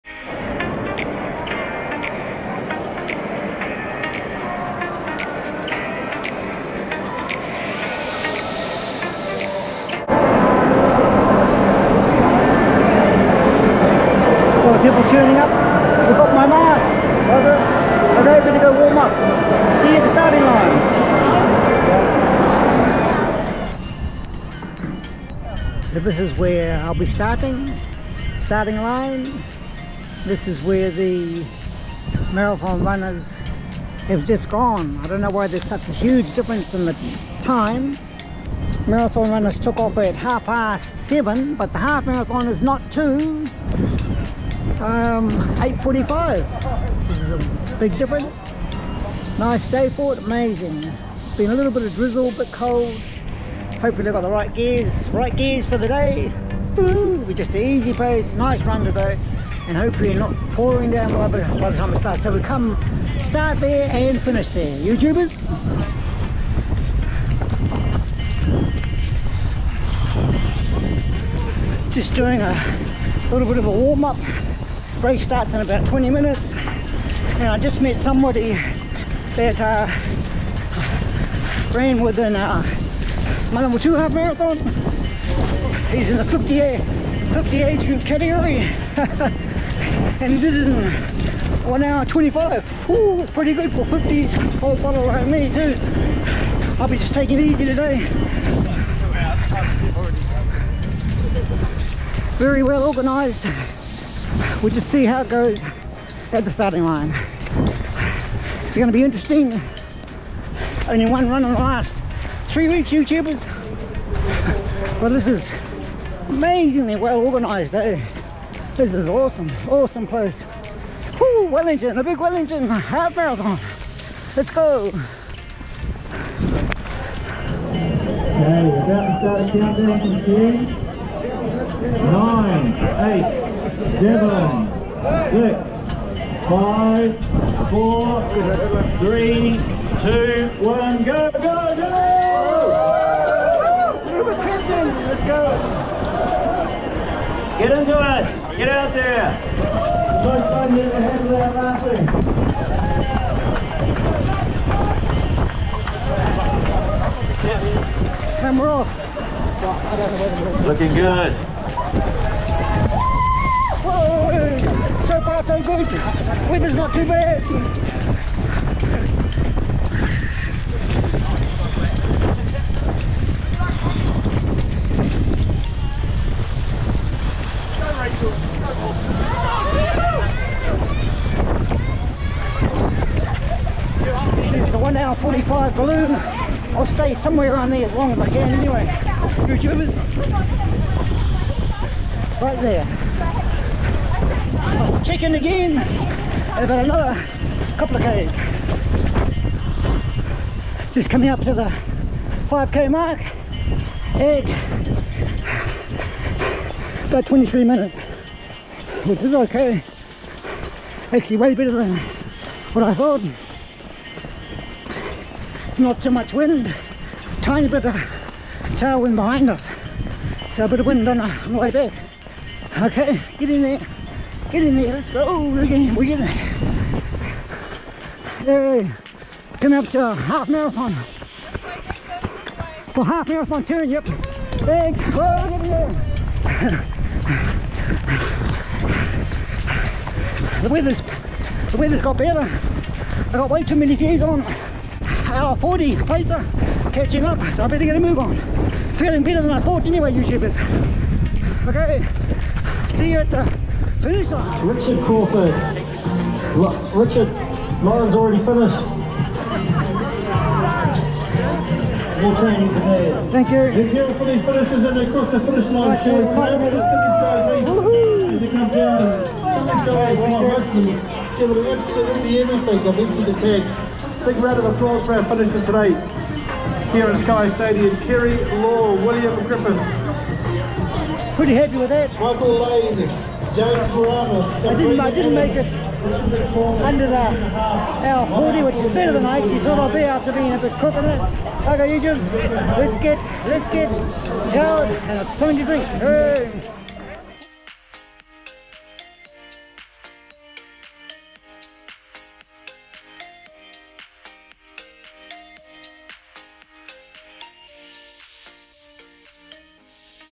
Wellington half marathon 2022
This is my 2nd half marathon this year, only had one run in the last 3 weeks due to injury & was a bit crook last week, paid for this race 2 years ago, took it easy just to see where my fitness is at the moment, thanks for stopping by, don’t forget to subscribe 😀